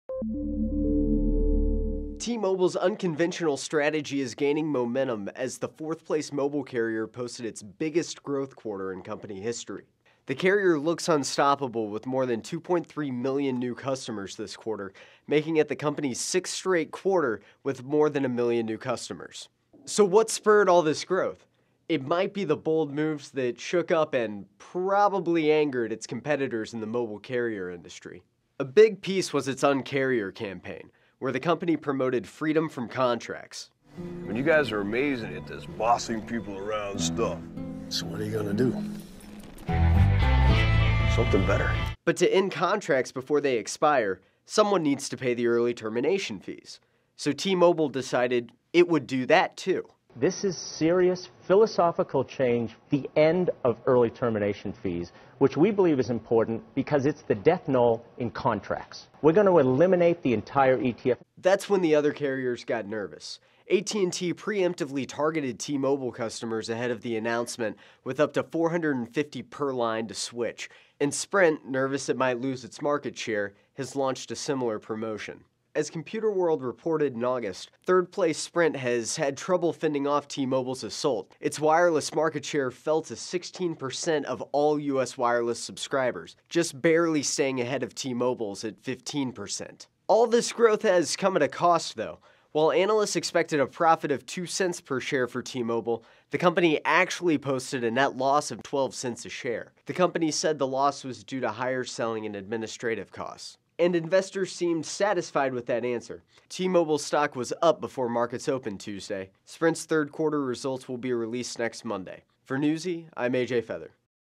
T-Mobile Sees Biggest Growth Quarter In Company History
T-Mobile's unconventional strategy seems to be working as the company gains momentum and market share.

Image via: Getty Images / David Becker